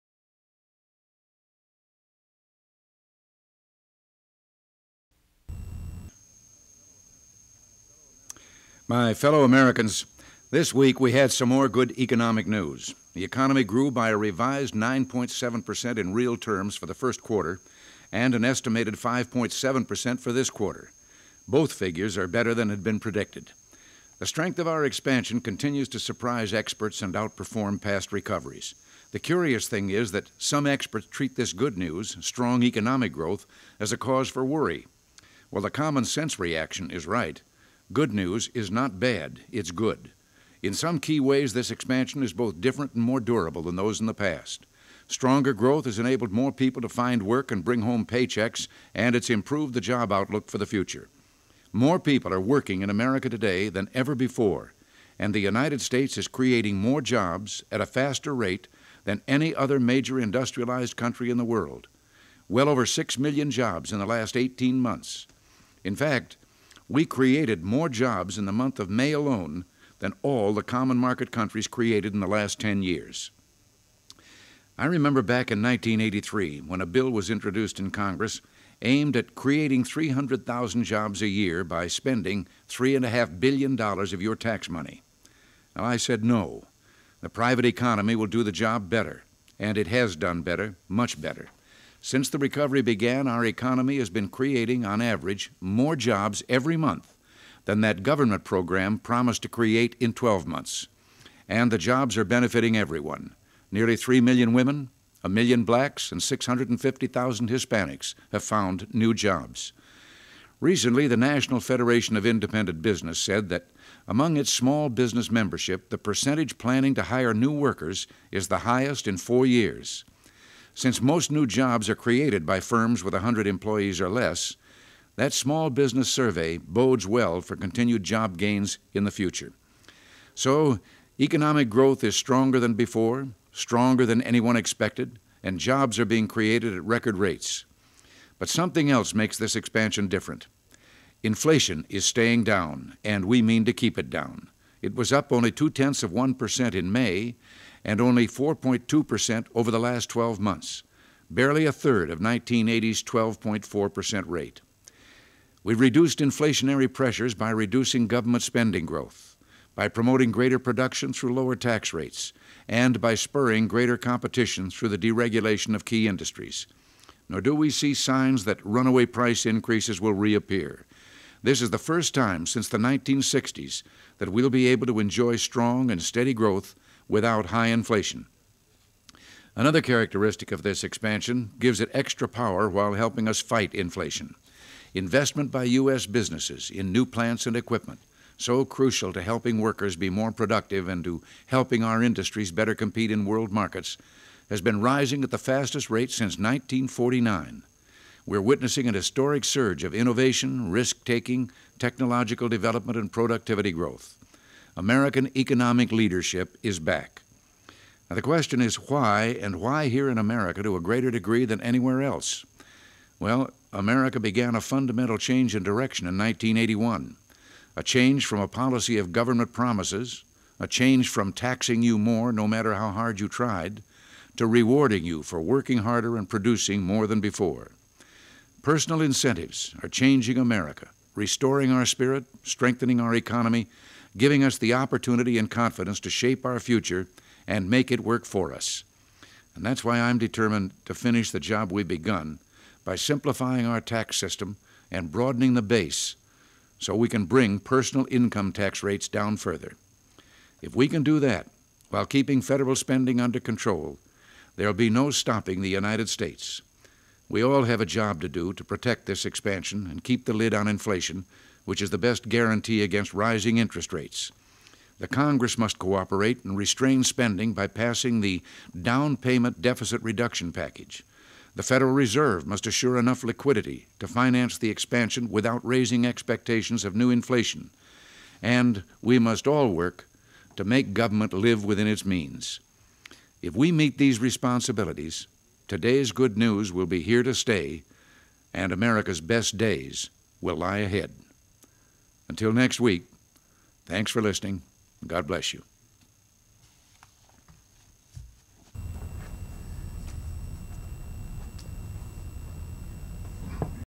President Reagan Radio Address to the Nation on the Economy at Camp David, Maryland on June 23, 1984
Full Title: President Reagan Radio Address to the Nation on the Economy at Camp David, Maryland on June 23, 1984

Access Restriction(s):Unrestricted
Use Restriction(s):Unrestricted